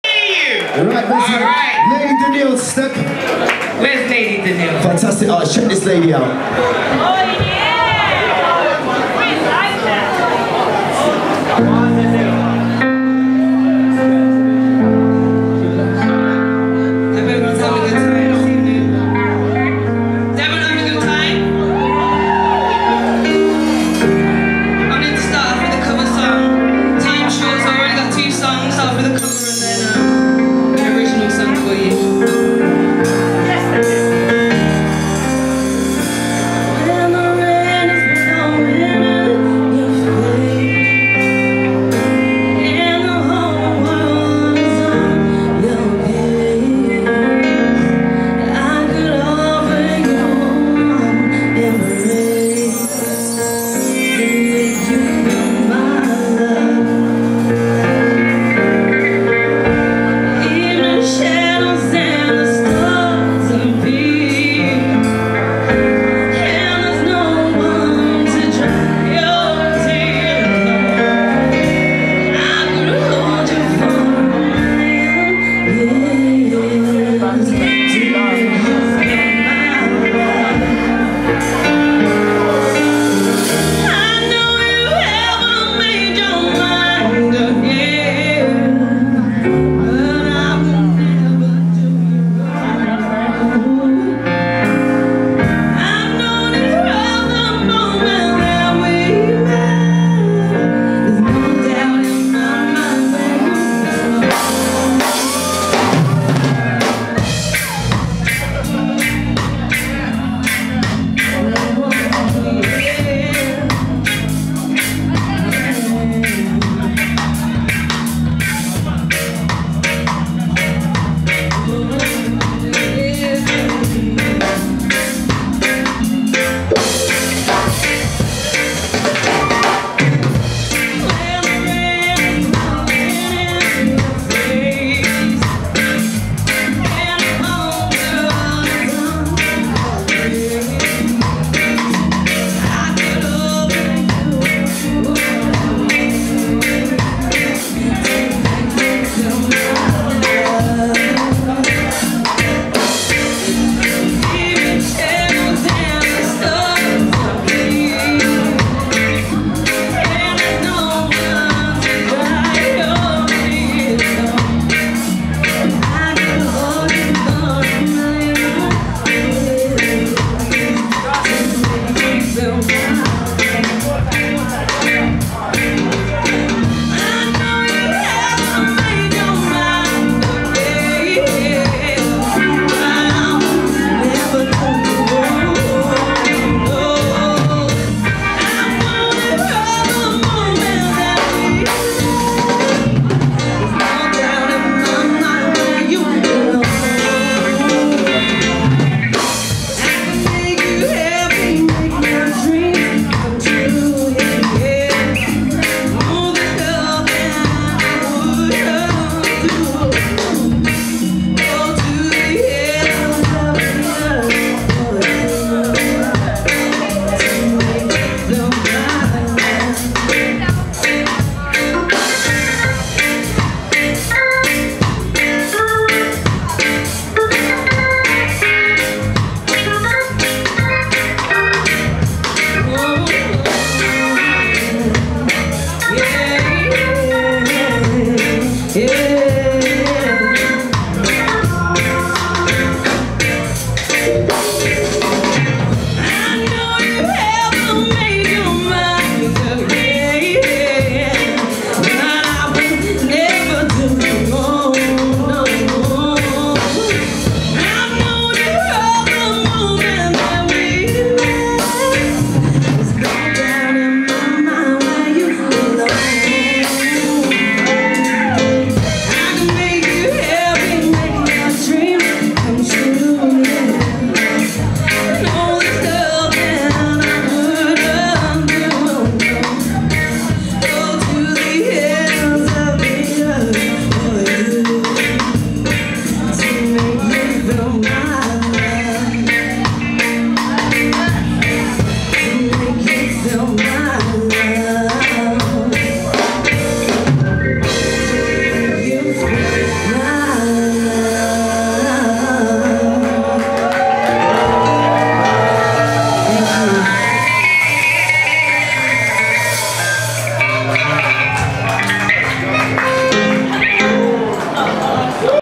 City Link up Britain's got reggae 9Jul15 Pt 9 (Danieal)
City Link up presents 'Britains got Reggae' - Heat 2 backed by the Maximum Heights band at the Hootananny, Brixton on 9th July 2015. Hosted by 5ive-0. Part 9. Danieal.